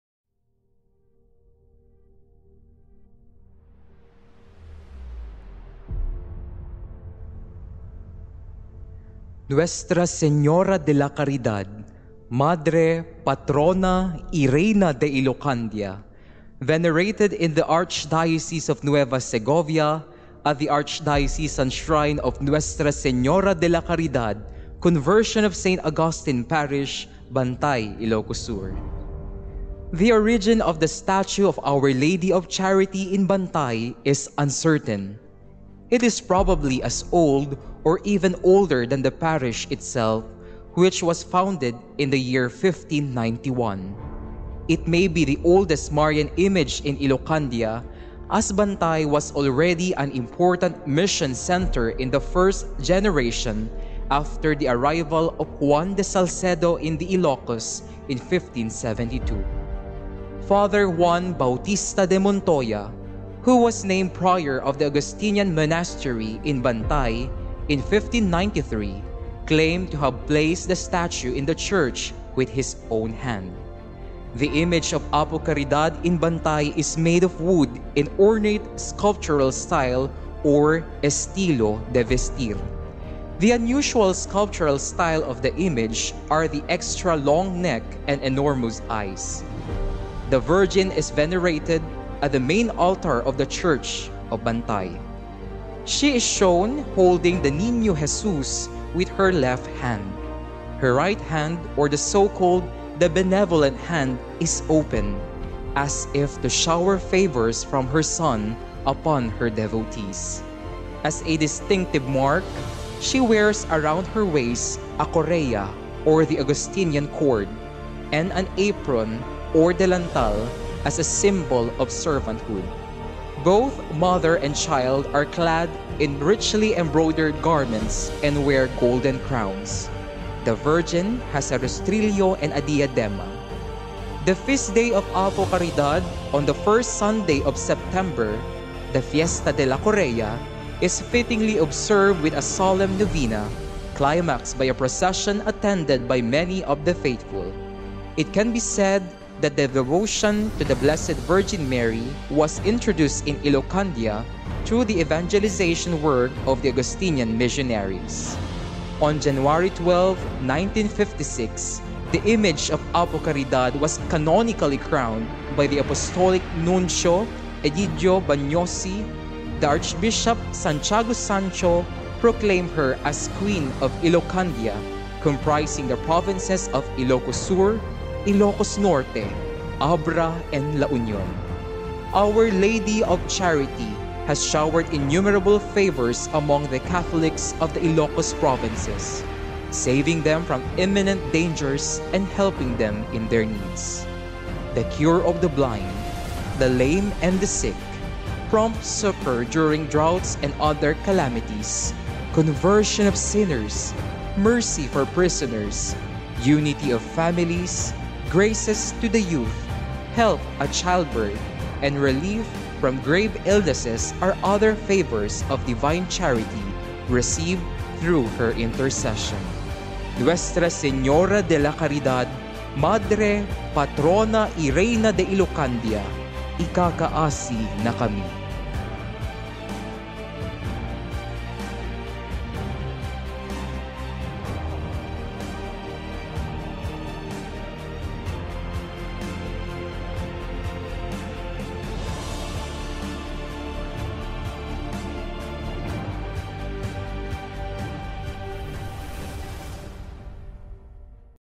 Brief History of the Image of Nuestra Señora de la Caridad
The statue of Our Lady of Charity is an old treasure of the church of Bantay venerated by the townsfolk since time immemorial. Historian Castro states that Fr. Juan Bautista de Montoya himself placed the statue in the church with his own hand. Gaspar de San Agustin calls this image very miraculous and of great veneration. That the convent and church were spared from destruction during the revolt of Malong in 1661 has been attributed by the people to the intercession of Our Lady of Charity, a miraculous event considering that the town was looted and many of the townspeople killed. The town became famous again during the Silang uprising in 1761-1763 when the protection of the image of Our Lady of Charity became evident once again. According to original Spanish sources, Bishop Ustariz was then imprisoned at the Augustinian convent, together with other priests, all of whom had been ostensibly “summoned by Diego Silang to discuss the defense against the British!” Silang had them imprisoned for several months until a Spanish mestizo, Miguel Vicos, killed him.
 The image of Our Lady of Charity could possibly be the oldest Marian image in Ilocos. It measures 50 inches tall. It is made of wood, except for the face and the hands which are made of ivory. The image was officially and solemnly crowned by Egidio Vagnozi, Apostolic Nuncio to the Philippines, on January 12, 1956. Years later, the late Bishop Santiago declared the church of Bantay as the Archdiocesan Sanctuary of Our Lady of Charity.